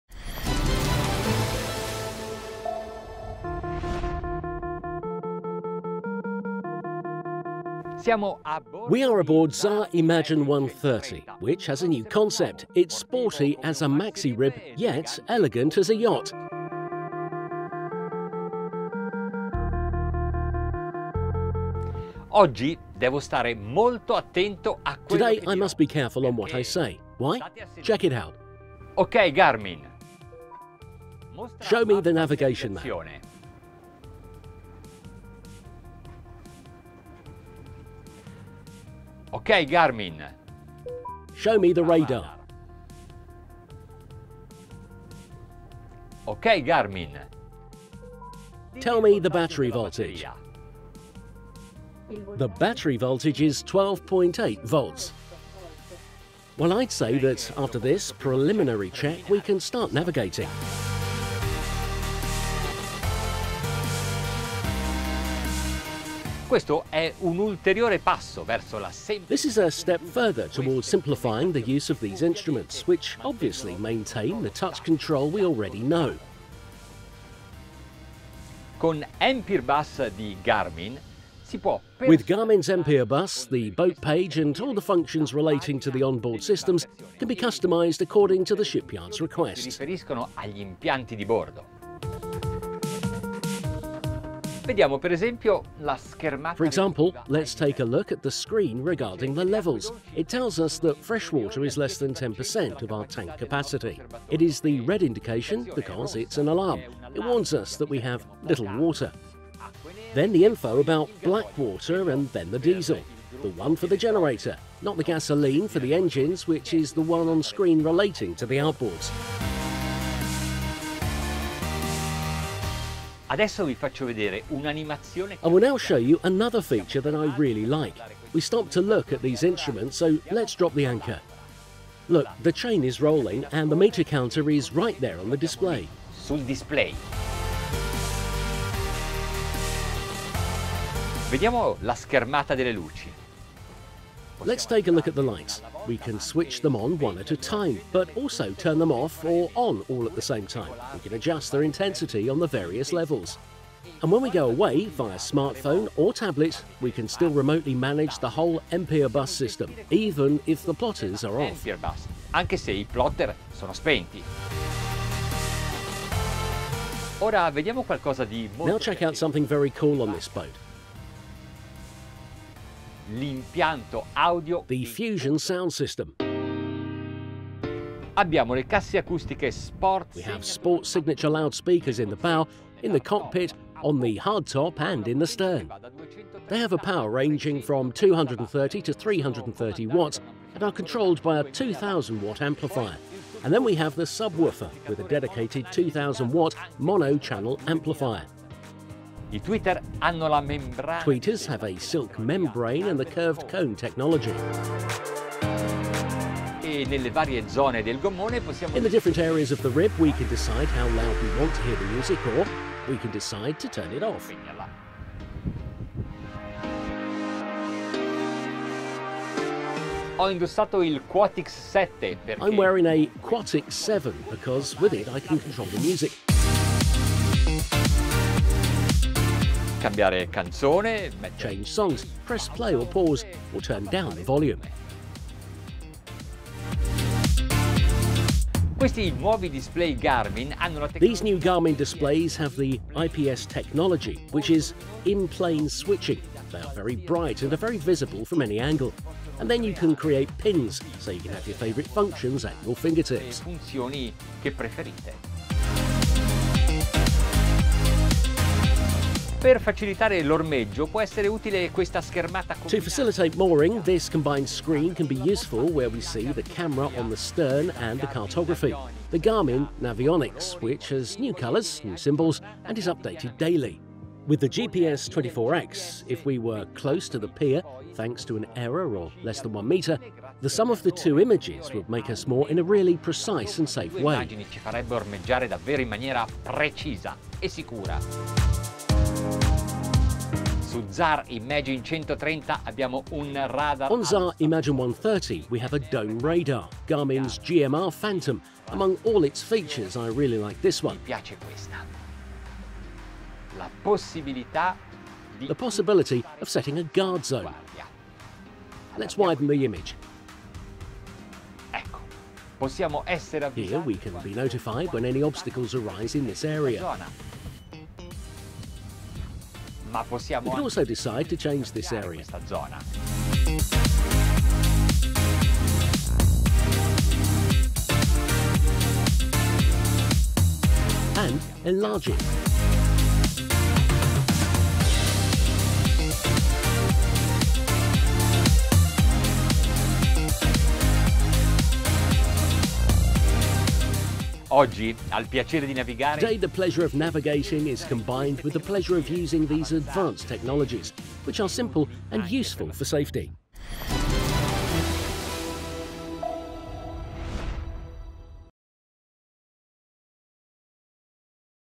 [ENG] GARMIN ELECTRONICS On Board Maxi Rib ZAR 130 - Integrated Systems Review - The Boat Show
By Maurizio Bulleri
Teh Boat Show 2022